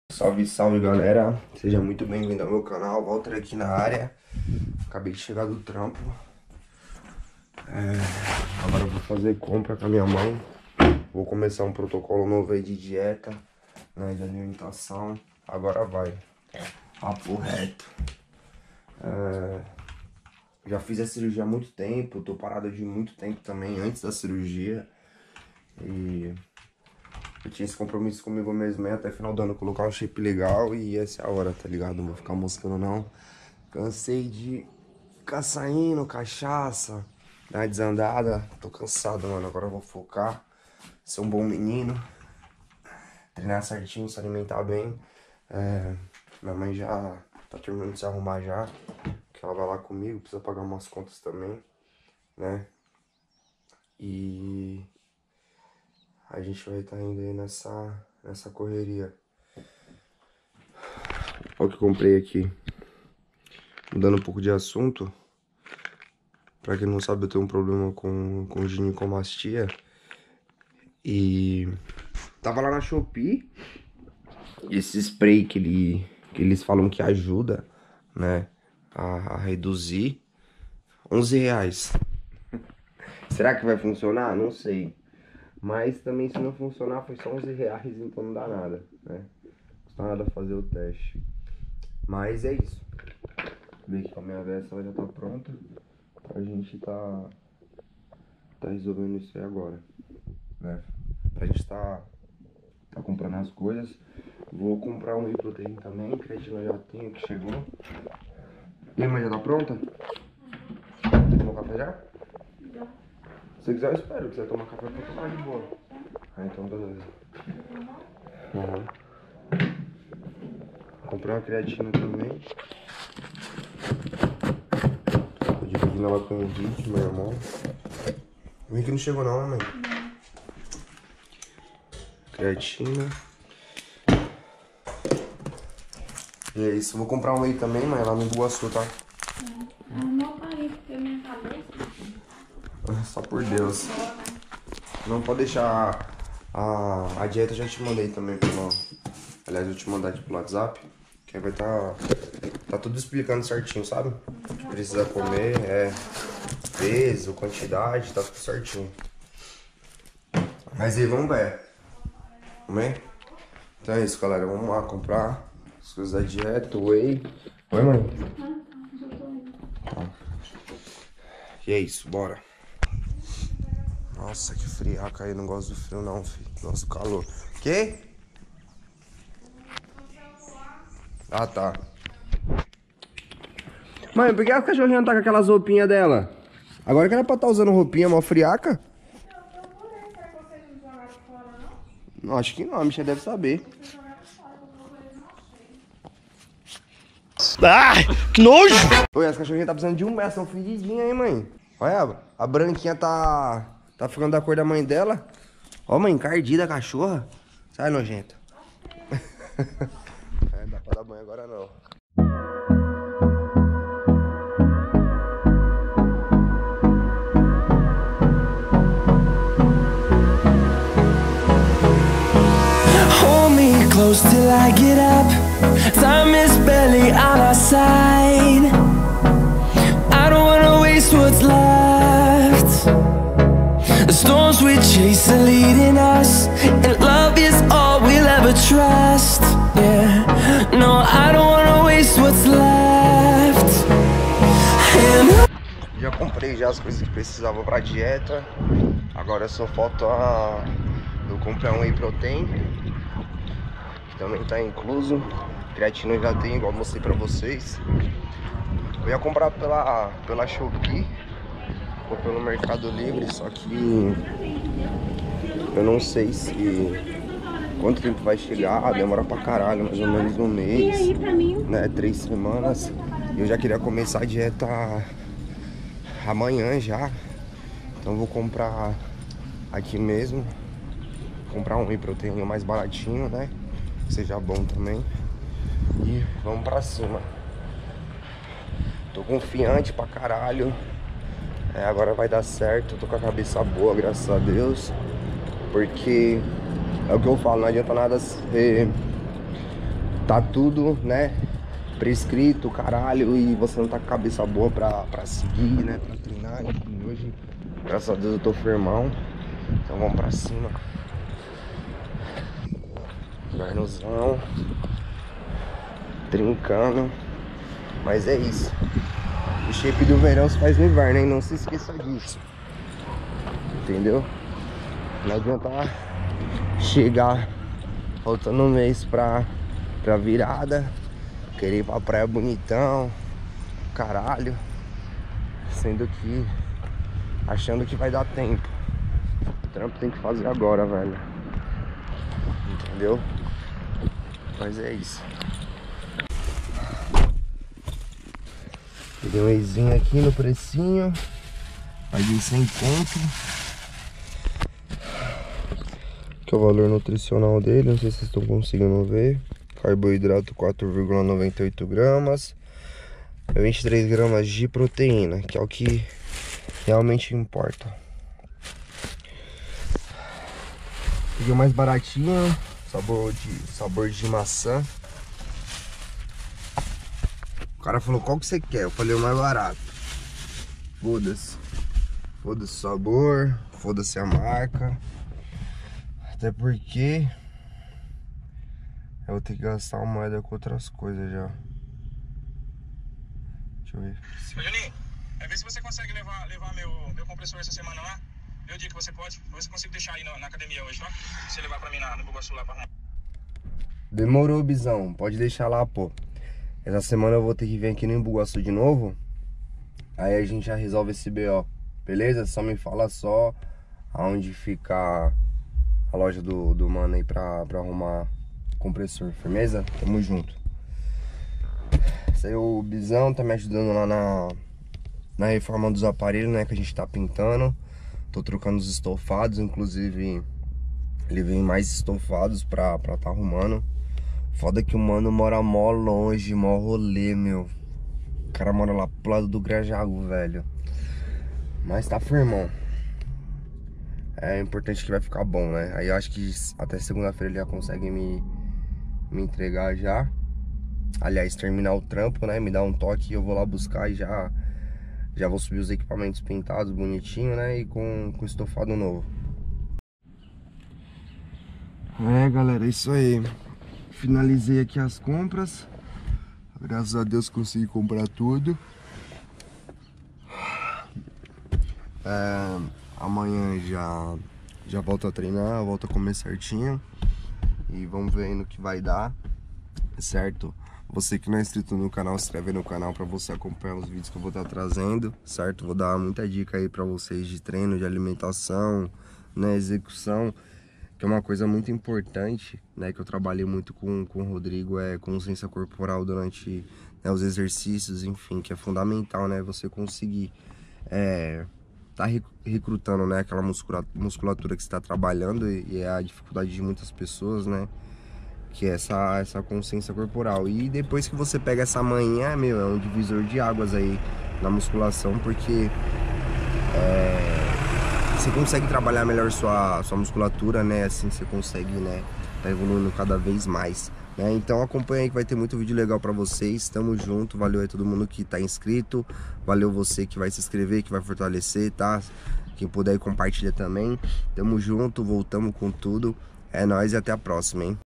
VOLTEI A FAZER DIETA | DEFININDO O CORPO EM POUCOS DIAS!
Tamooo de volta porr@@!
Nesse video vou fazer compras para minha dieta, vou definir o corpo em poucos dias! acompanhem... 🔥🐷